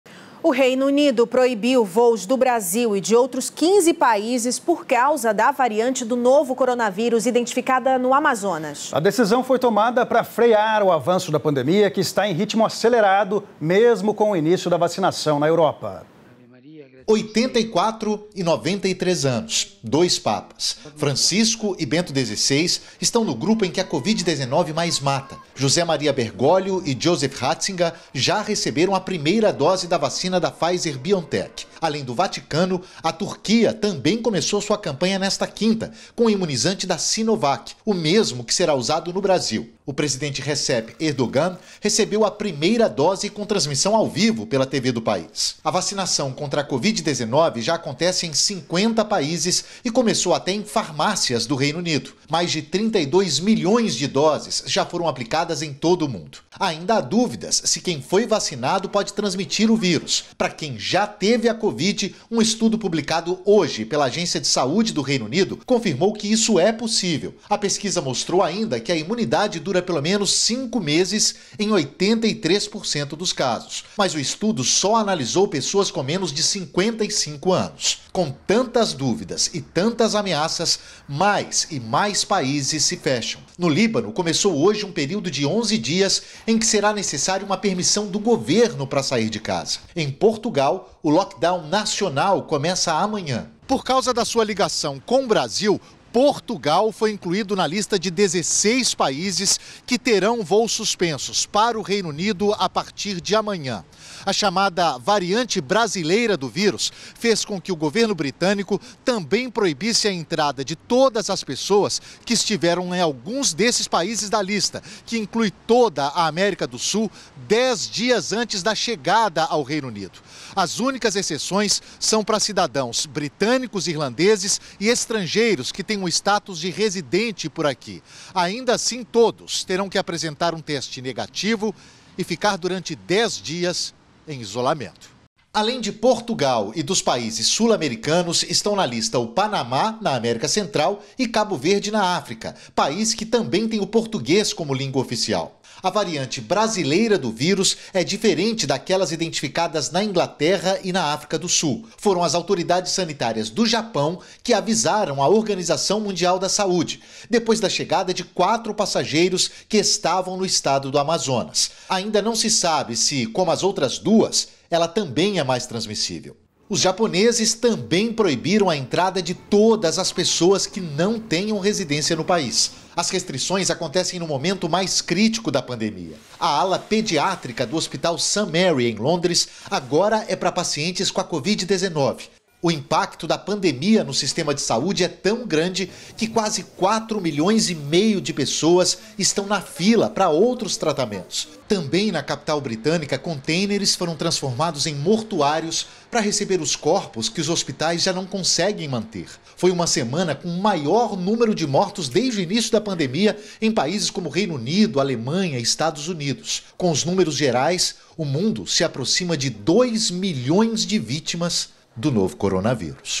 Covid-19: Reino Unido proíbe voos do Brasil devido a variante no AM | SBT Brasil (14/01/21)
O Reino Unido proibiu voos do Brasil e de outros 15 países por causa da variantes do novo coronavírus identificada no Amazonas. A decisão foi tomada para frear o avanço da pandemia, que está em ritmo acelerado mesmo com início da vacinação na Europa. Nesta quinta-feira (14), a Turquia começou a campanha vacinar em massa contra a Covid-19.